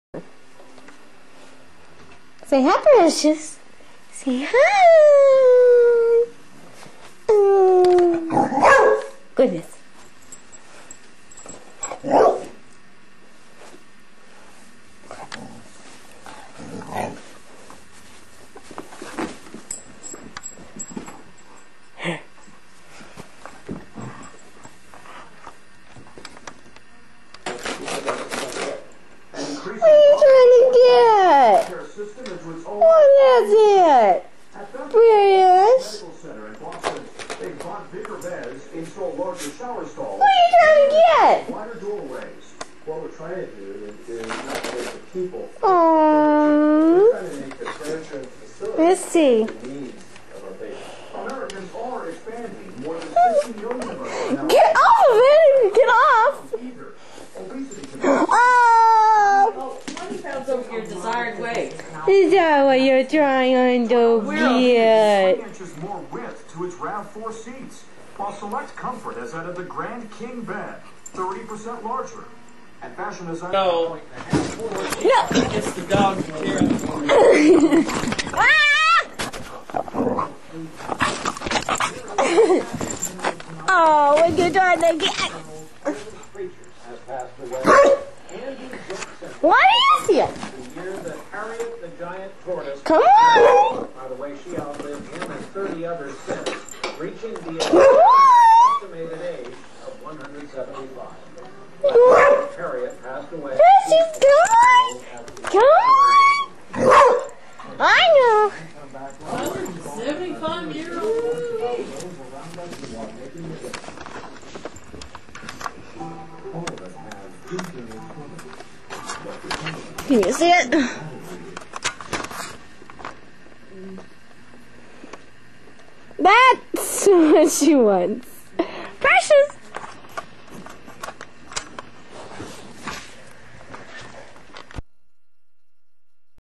My Dog
Really, really old video I found. This is my dog. she sounds evil but shes just playing around! lol that's me talking in the background oh and my dogs name is Precious~♥

0_______0 29,000 VIEWS?!! thanks?